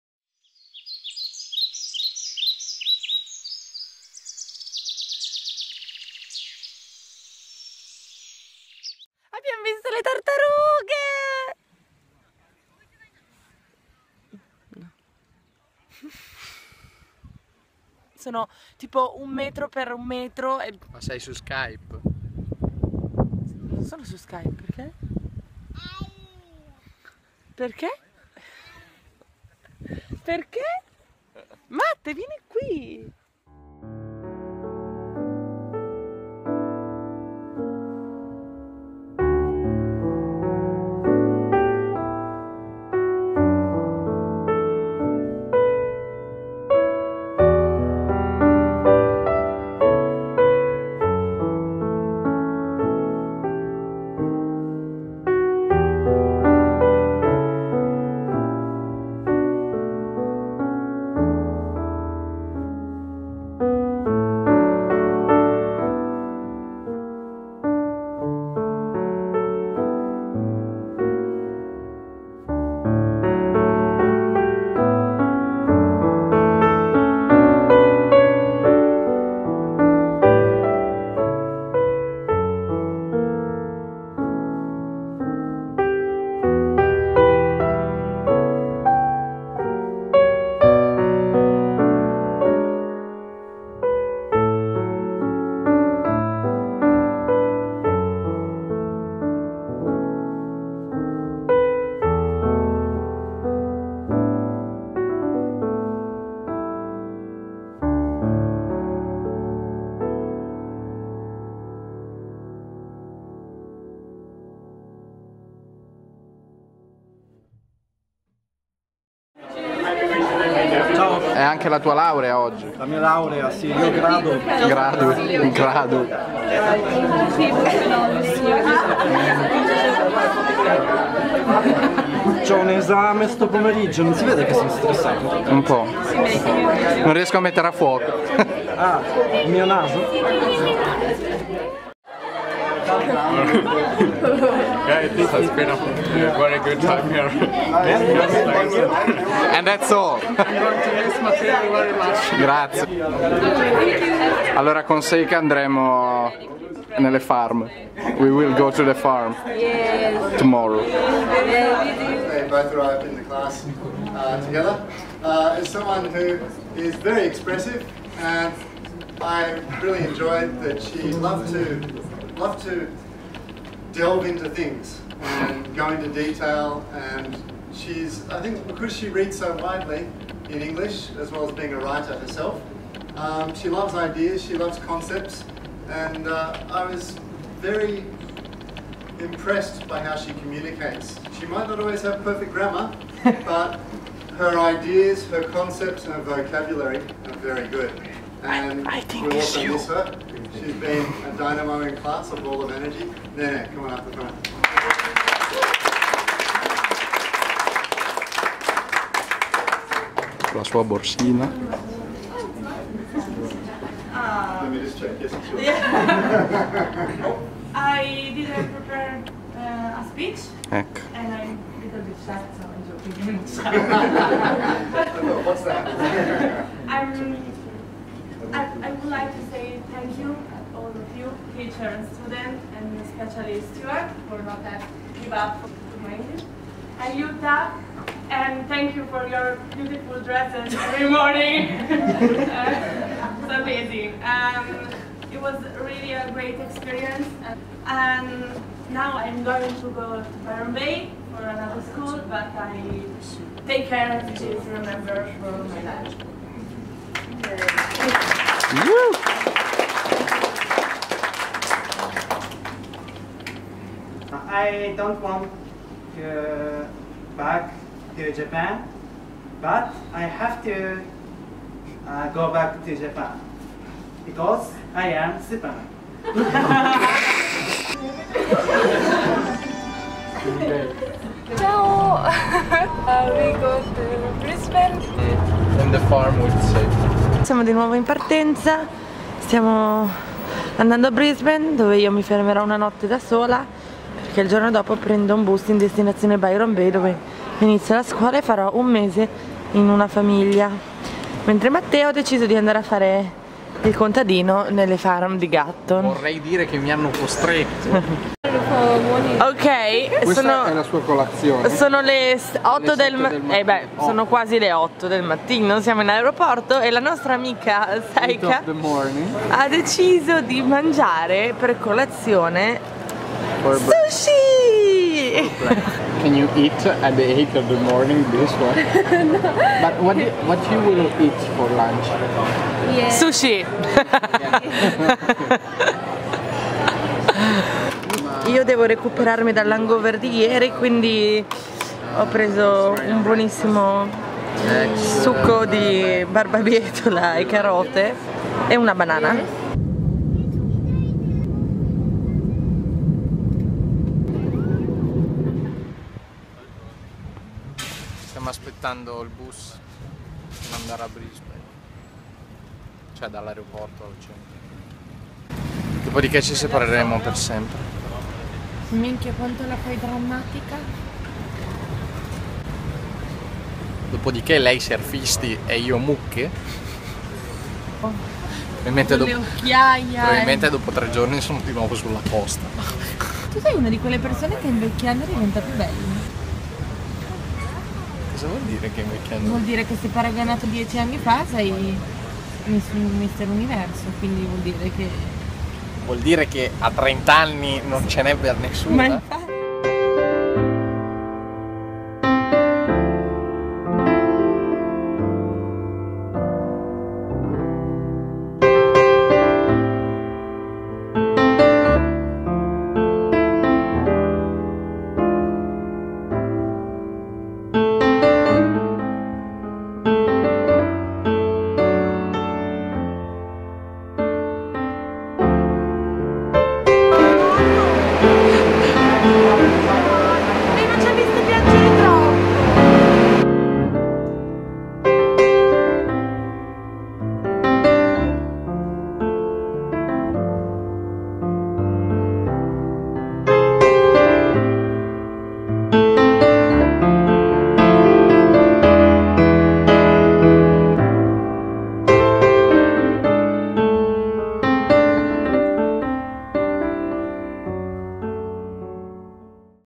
Roadtripping - Capitolo 11/La grande barriera corallina e l'ultimo saluto a Cairns.
Dalle fantastiche tartarughe della Green Island all'ultimo giorno in Cairns prima della nostra partenza in due diverse direzioni.